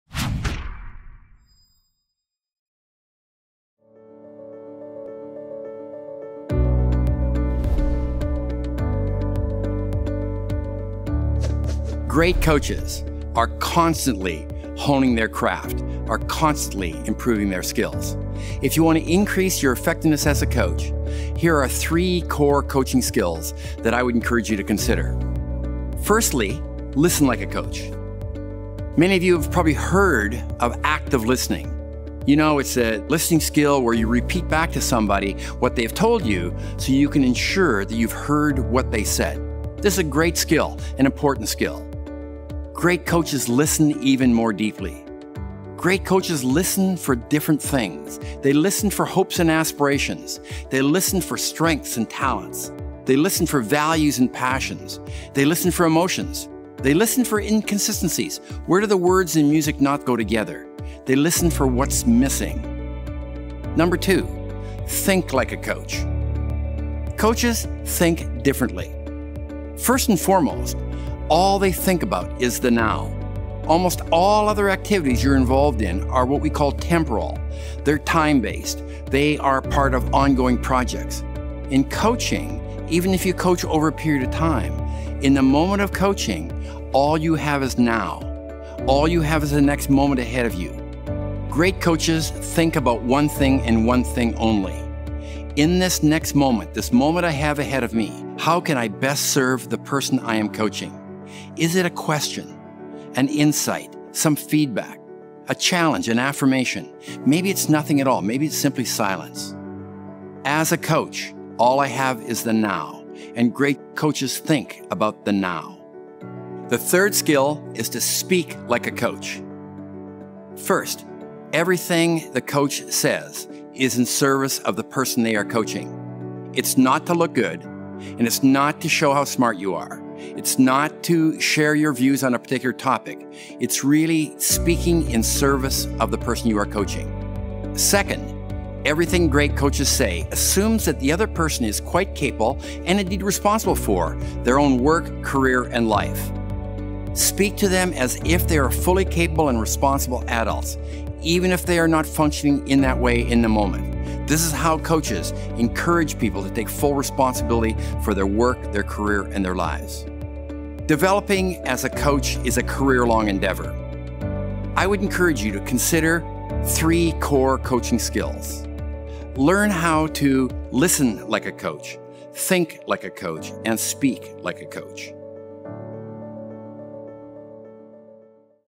The Three Core Coaching Skills
Nothing compares to coaching when it comes to helping people perform at their best and accelerate their careers. Individuals become energized (or re-energized) about their work, take full ownership of their performance and their careers, find and rejuvenate long-lost talents and make major shifts in their contribution levels. Coaching is by far the most effective tool at a manager's disposal. The problem is that today's organizations are extremely frenetic and managers face near limitless demands on their time. So much so, that when it comes time to coach, most managers don't know where to start.